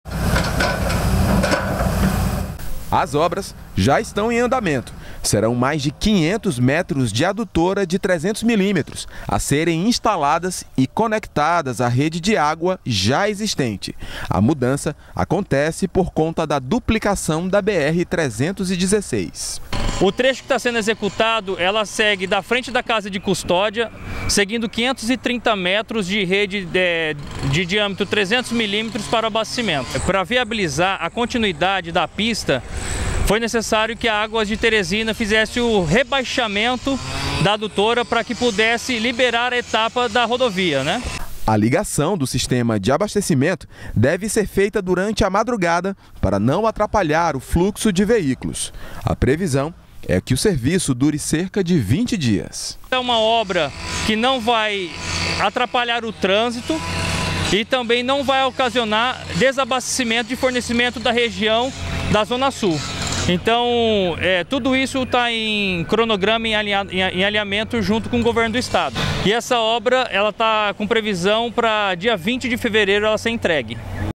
Adutora da zona sul será remanejada
Uma adutora localizada no Sul de Teresina, da empresa Águas de Teresina, vai ser remanejada. A mudança deve ocorrer em função da obra de duplicação da BR-316. A reportagem é de André dos Santos e Volmar Cardoso, da TV Assembleia.